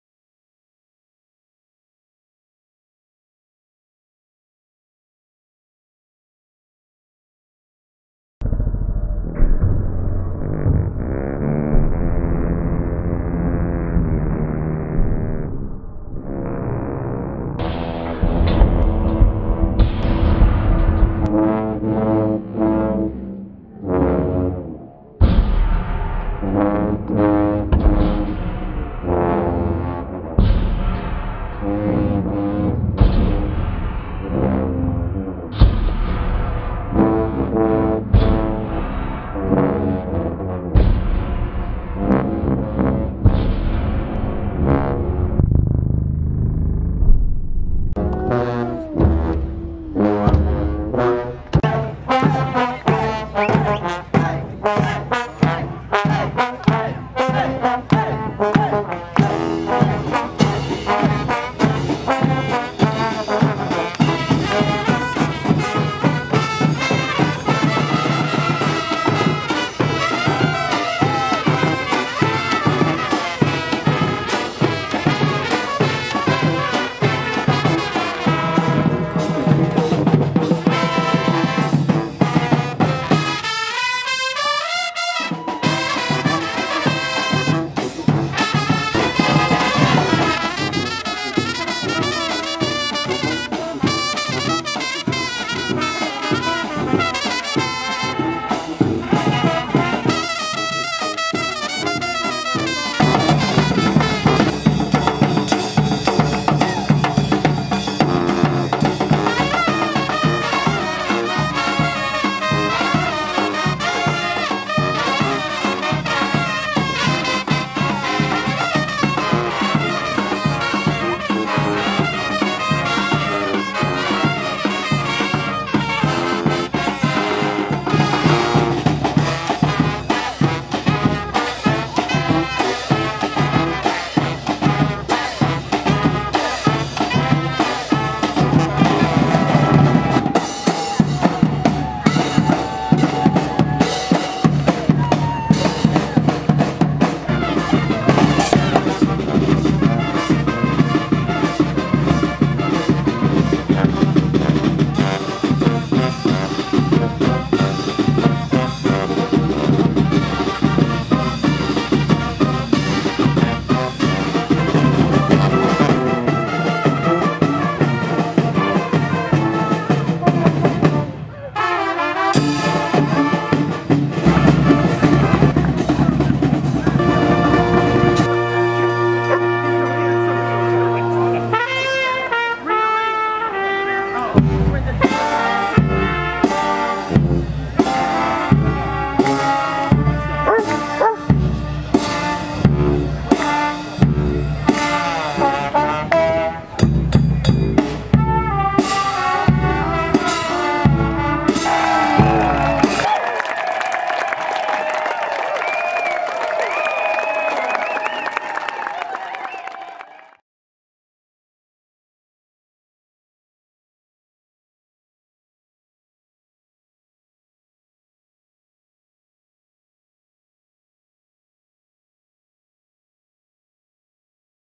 What Cheer Brigade Energizes Crowd at Waterfall Arts 10th Birthday Bash!
Waterfall Arts:
Creating community in harmony with nature through the transformative power of the arts. Belfast, Maine